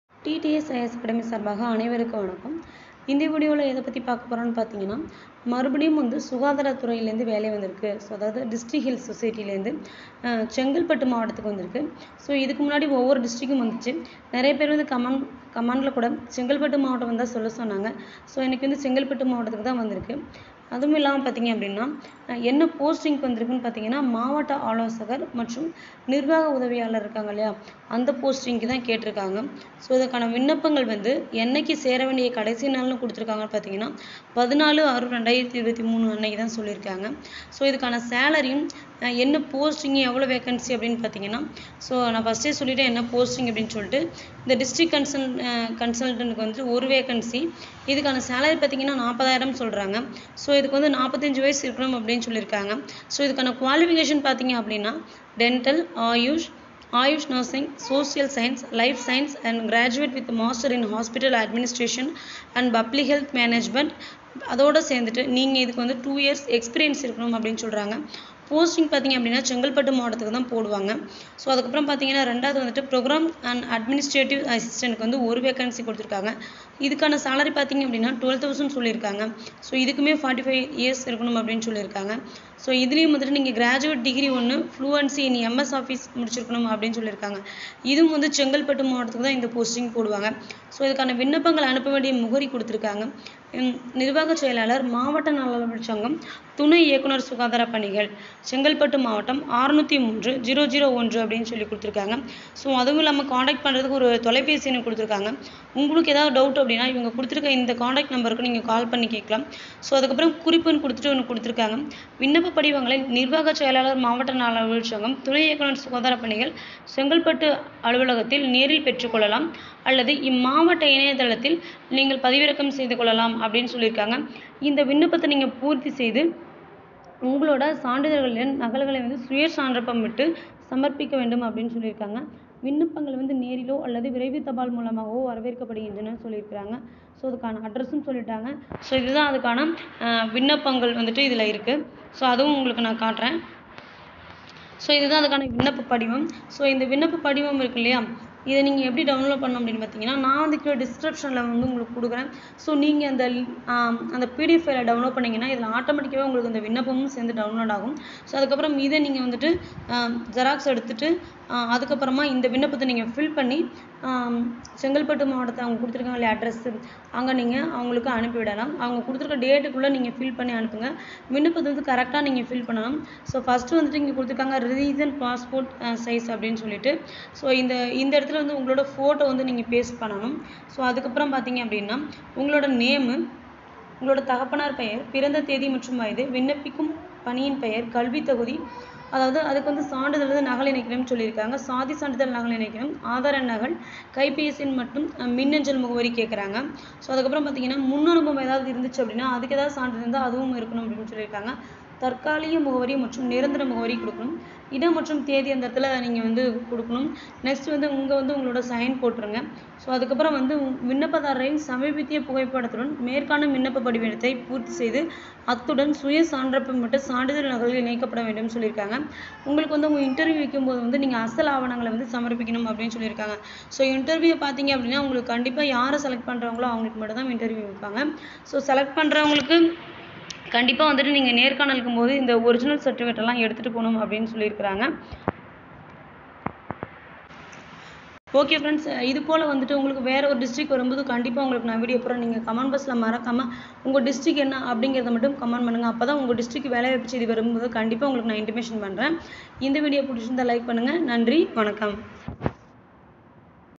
😍😍 MS Office முடிச்சி இருந்தா போதும்/ இந்த வேலைக்கு நீங்க அப்ளை பண்ணலாம்
சுகாதார துறையில் வேலை